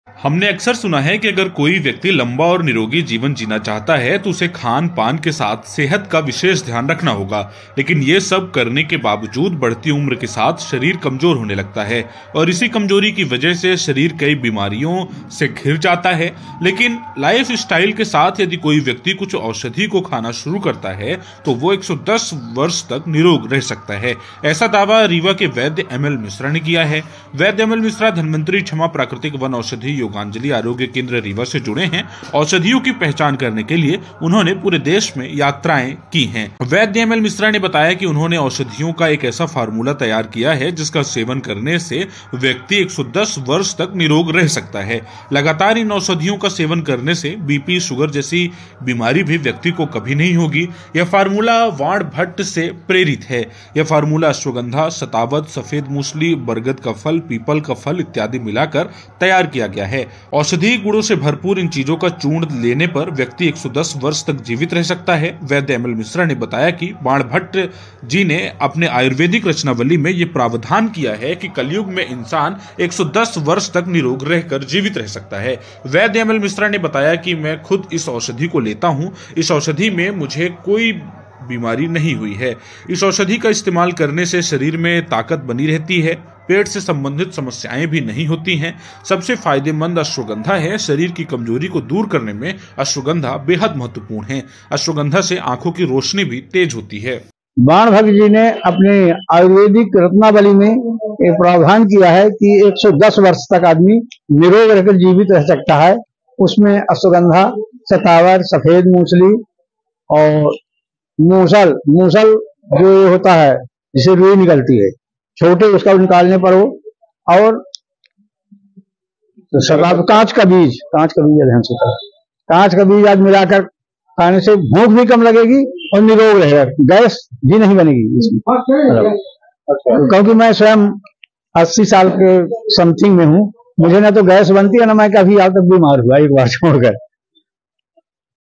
Health News : इस औषधि के इस्तेमाल से 110 साल तक आप रहेंगे निरोग!
We have often heard that if a person wants to live a long and healthy life, he will have to take special care of his health along with eating habits. But despite doing all this, the body starts becoming weak with increasing age.
हमने अक्सर सुना है कि अगर कोई व्यक्ति लंबा और निरोग जीवन जीना चाहता है, तो उसे खान पान के साथ सेहत का विशेष ध्यान रखना होगा. लेकिन ये सब करने ने बावजूद बढ़ती उम्र के साथ शरीर कमजोर होने लगता है